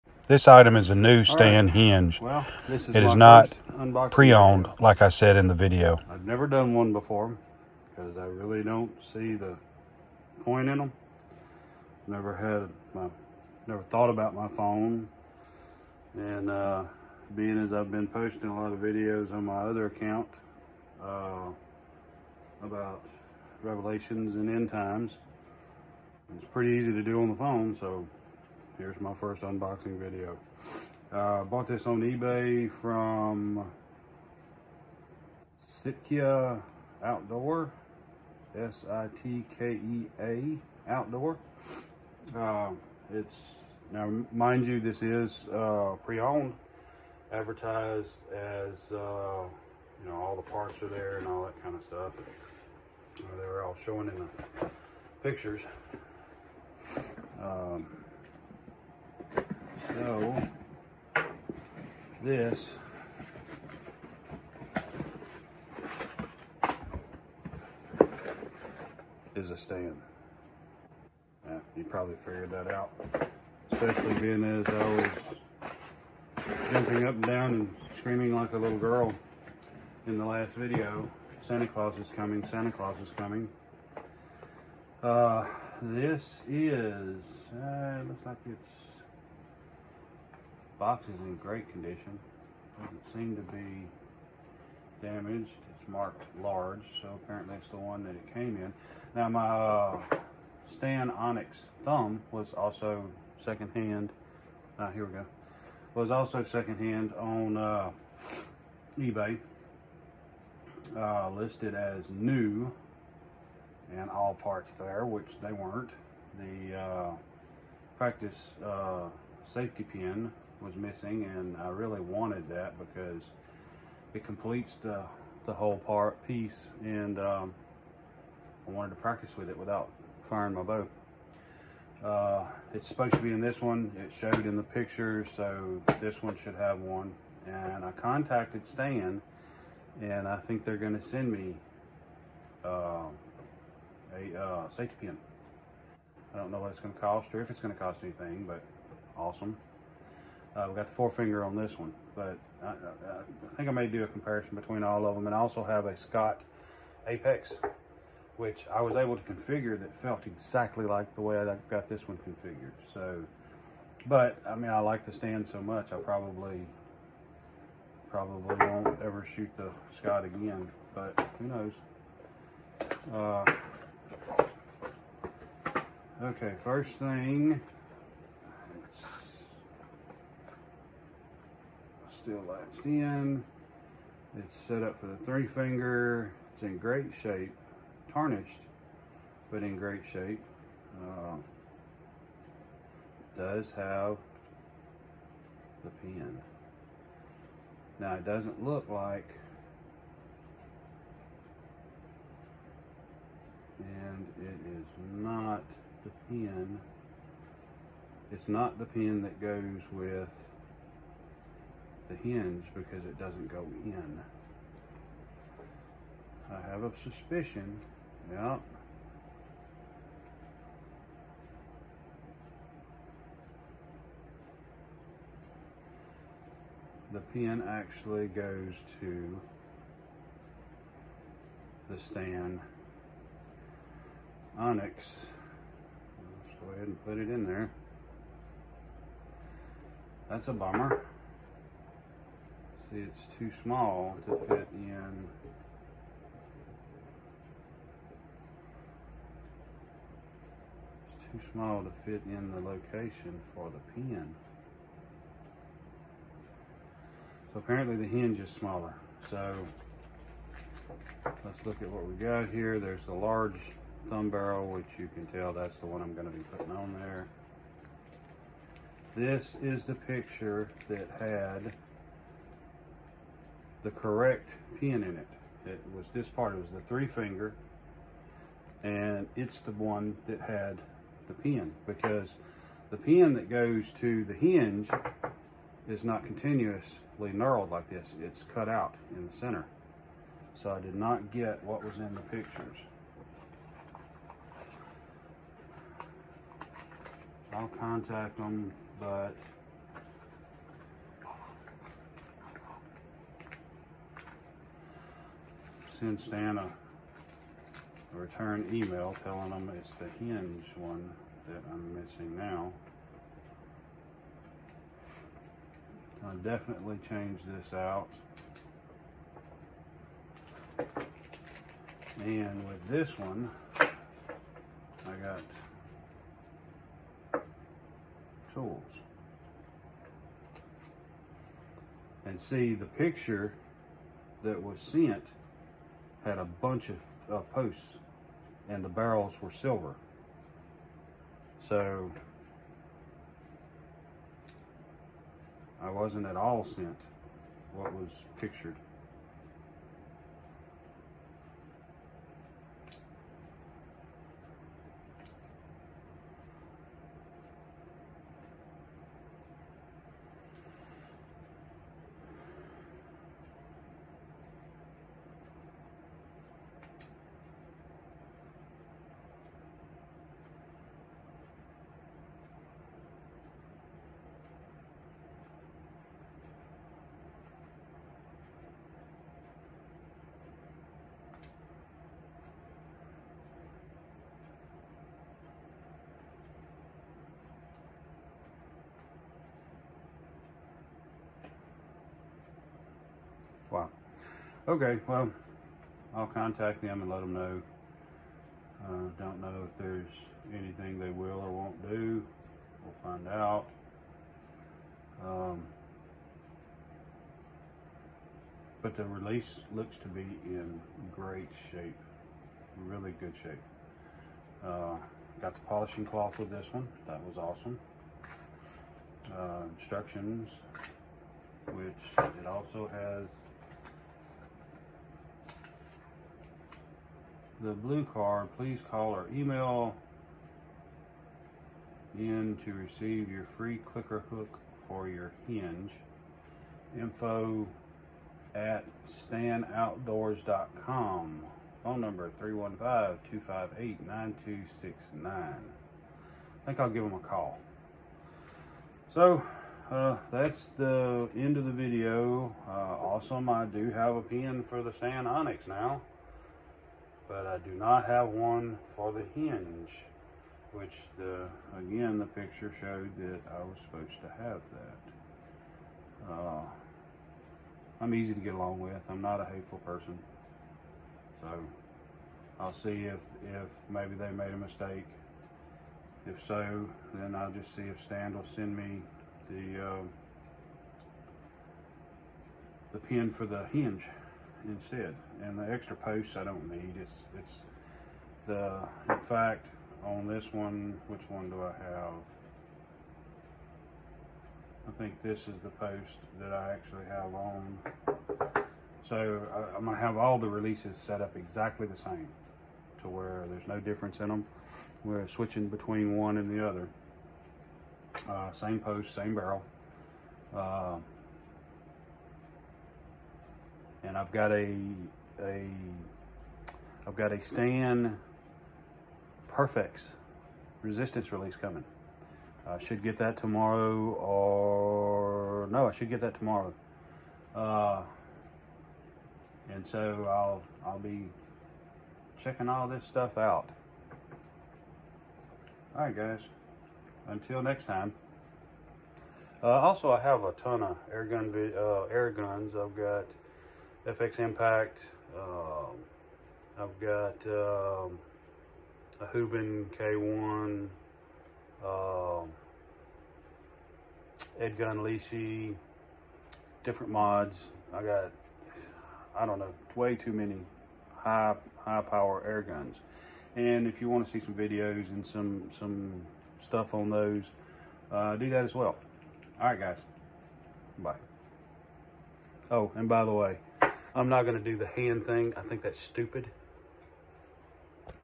Stan hinge unboxing
Im apparently not very good at filming a unboxing. Ill work on doing a review on my higher end releases after i get the perfex resistance in and a few shots down range with them.